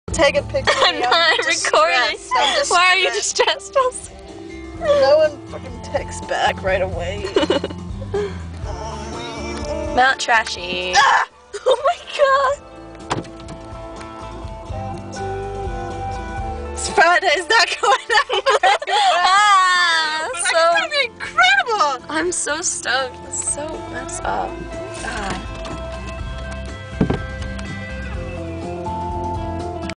March 9 - Waiting at Mt. Trashy with Chelsea
Waiting for JP & Matt at Mt. Trashmore